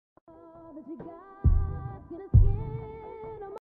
Comment assumptions about me! I’m bored lol
Everyone else is doing this so I thought that I would too lol😂 also I’m so obsessed with the most recent episode of Canada’s Drag Race! I loved literally all of the verses!